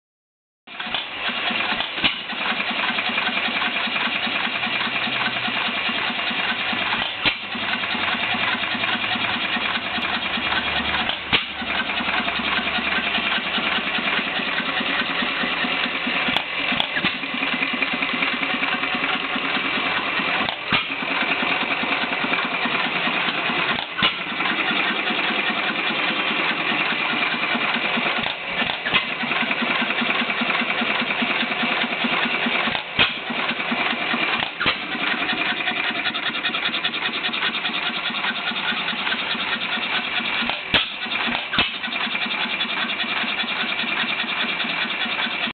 Myrick Machine Co., 4 hp oil field gas engine 1914
Made approximately 1914. I removed this engine from its original installation in November, 2009. I used to pump the gas well this was on back in the mid 1980s.This video footage is of the first run since being mothballed in 1997. It was made by Myrick Machine Company, Olean, NY.